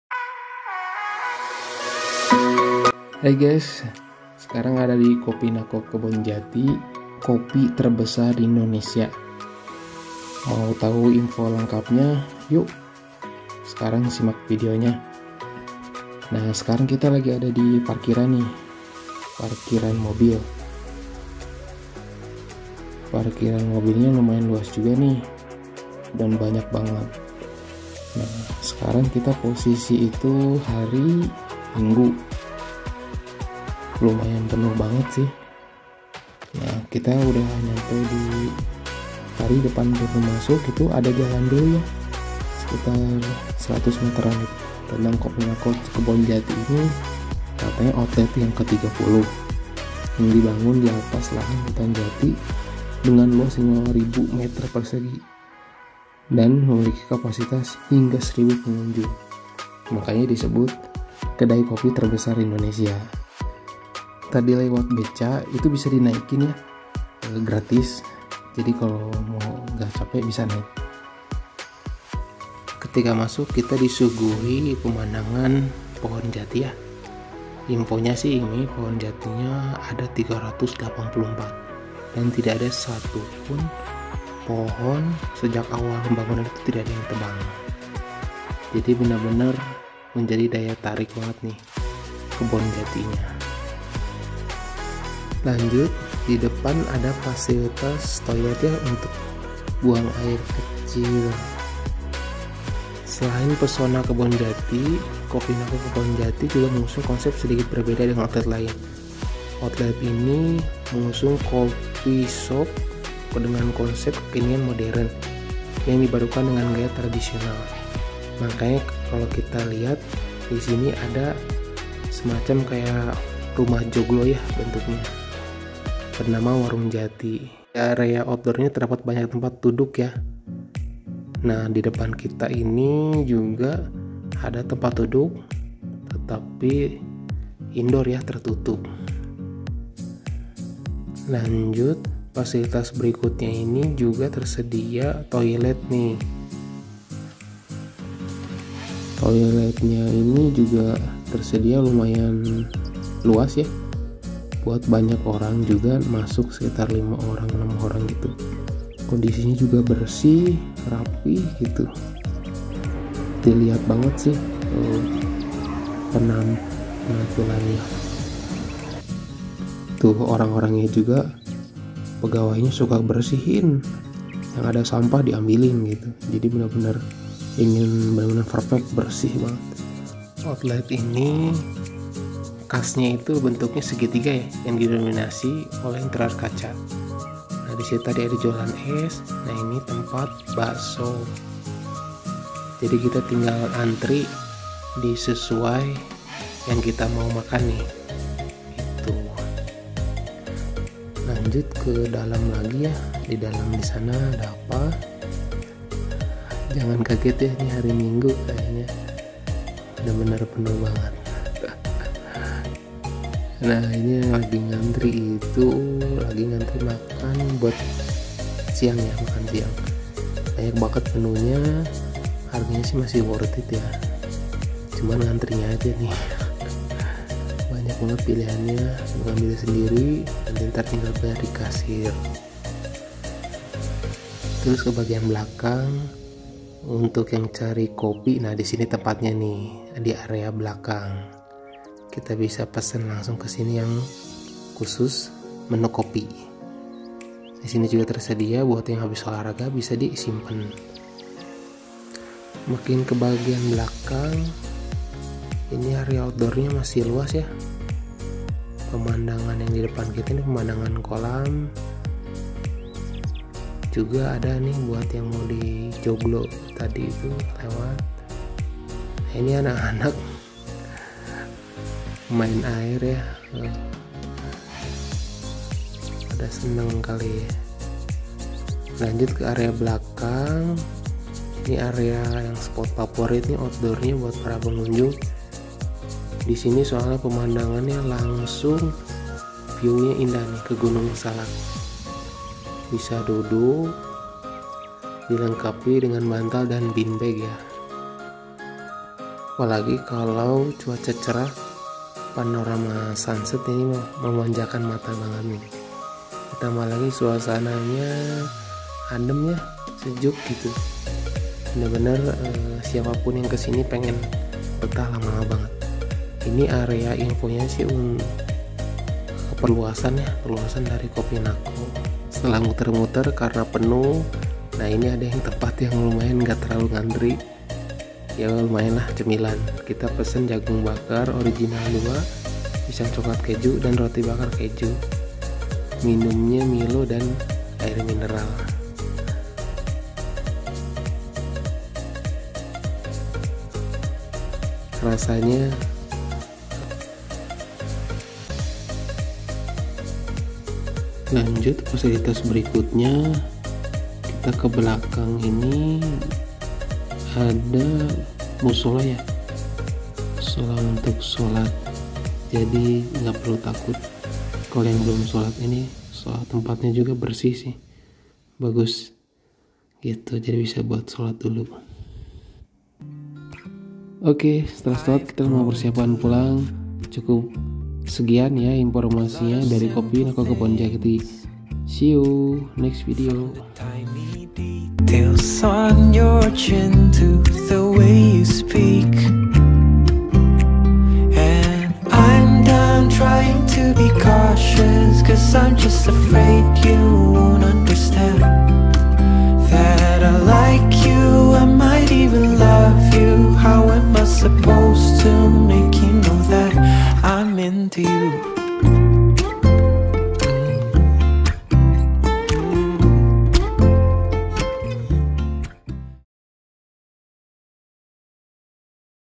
Wisata Kuliner, Kopi Nako Kebon Jati Bogor Terluas, Terlengkap
Kalau ingin nongkrong, makan siang, atau ngemil sambil healing, kopi Nako Bogor bisa jadi alternatif nya nih. Pemandangannya terbaik yaa, situasi udara nya sejuk, adem karena banyak pohon jati, ditambah banyak pilihan menunya dan makanannya enak-enak.

Yuk bisa kunjungi ke Kopi Nako Kebon Jati di alamat Jl. Repormasi Kp.Legok Gadog No.Rt.09/06, Pasir Angin, Kec. Megamendung, Kabupaten Bogor, Jawa Barat 16770.

Gampang banget tinggal ikuti google maps, karena titiknya sudah sesuai banget😊